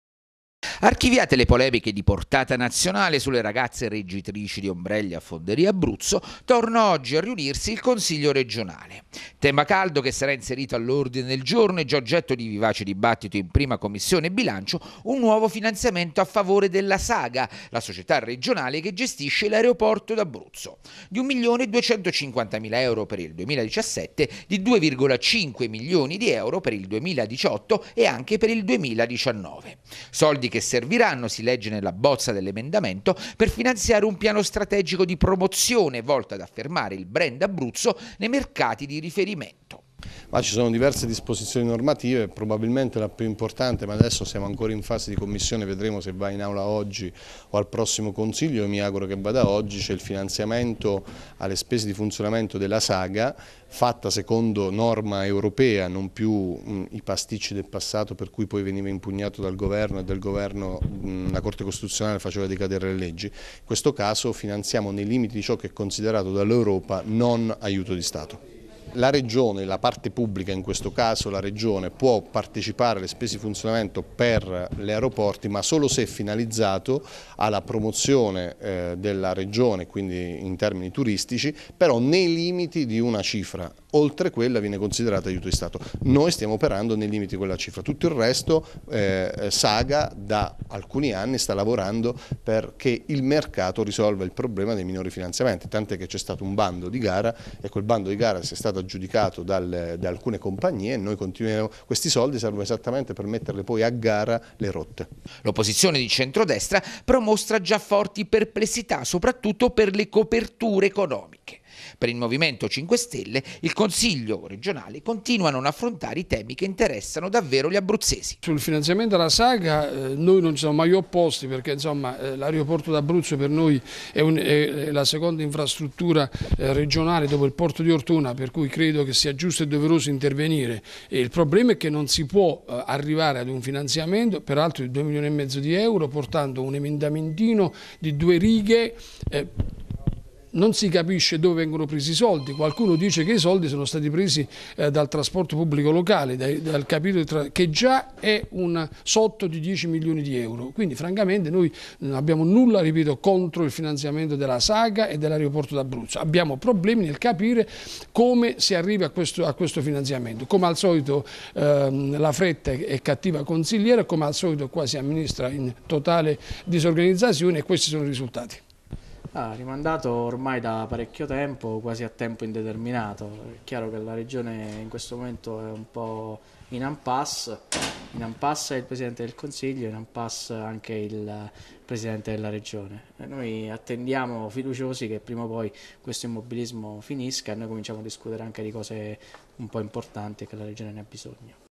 Il Centrodestra in Regione polemizza sui fondi alla Saga
Fondi fuori sacco alla Saga, società di gestione dell'aeroporto d'Abruzzo di Pescara.. Per l'opposizione di centrodestra in Consiglio Regionale però non ci sono certezze sulla copertura economica della variazione in bilancio